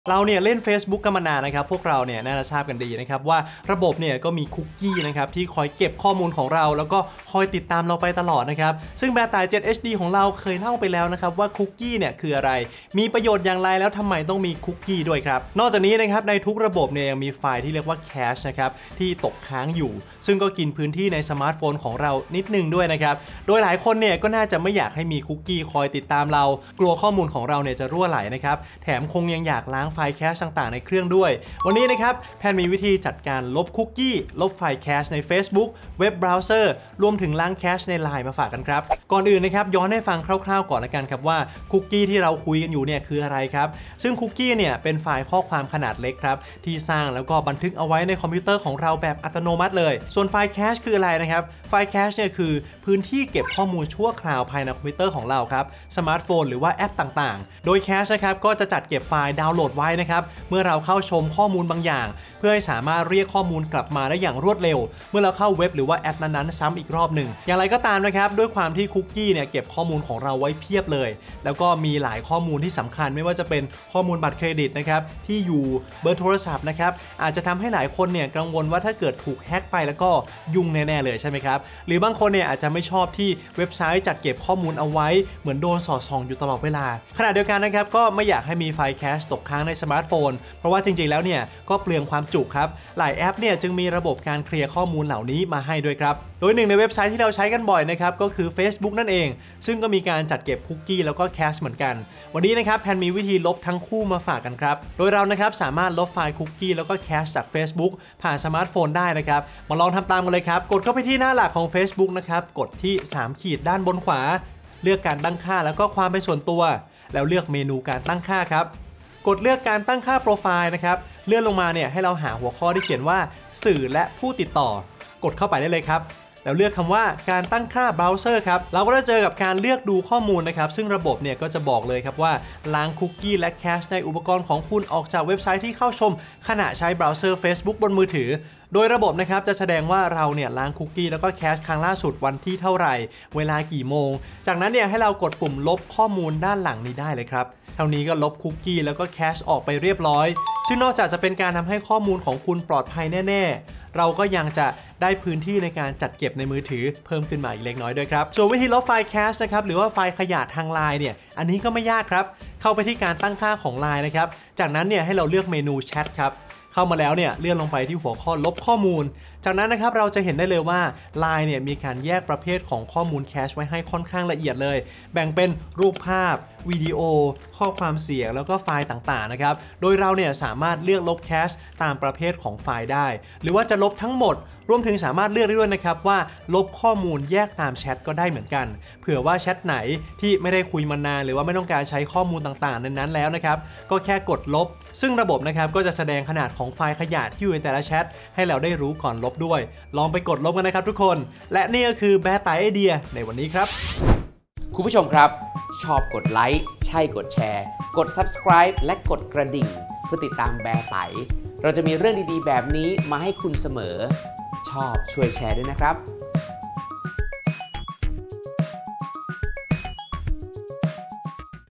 ลบไฟล์ขยะ ไฟล์แคช Facebook และ Line
เราเล่น Facebook กันมานาน น่าจะทราบดีแล้ว ว่าระบบจะมีคุกกี้ที่คอยเก็บข้อมูลของเรา และคอยติดตามเราไปตลอด นอกจากนี้ ในทุกระบบจะยังมีไฟล์แคชที่ตกค้างอยู่ ซึ่งกินพื้นที่ในสมาร์ตโฟนเรานิดหน่อยด้วย โดยหลายคนก็น่าจะไม่อยากให้มีคุกกี้คอยติดตามเรา กลัวข้อมูลส่วนตัวจะรั่วไหล แถมอยากล้างไฟล์แคชต่าง ๆ ในเครื่อง คลิปนี้เรามีวิธีจัดการลบคุกกี้ ลบไฟล์แคช ใน Facebook เว็บเบราว์เซอร์ รวมถึงล้างแคชในไลน์มาฝากกัน

LINE:           @beartai